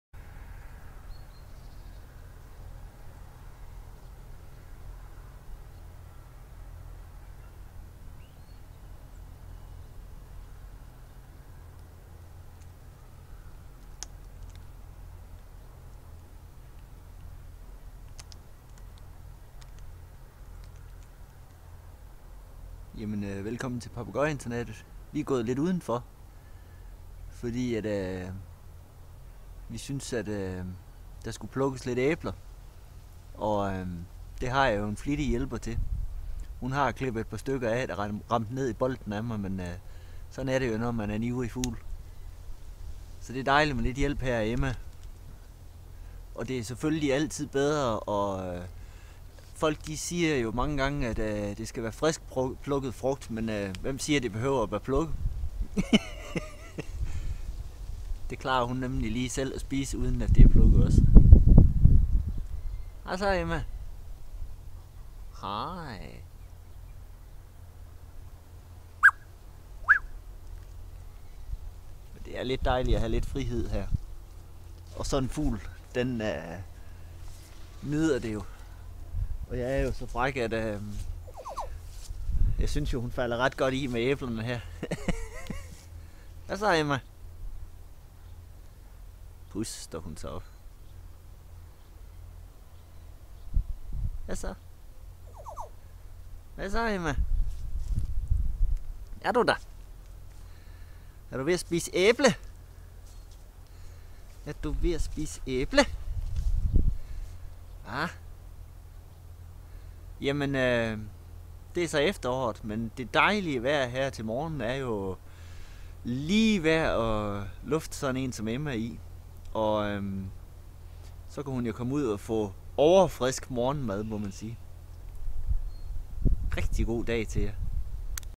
Papegøjeinternat : Emma grå jaco Papegøje på Æblerov :-)
I dag var papegøjen Emma med ude og nyde det gode vejr, og hun fik smagt på Ingridmarie æblerne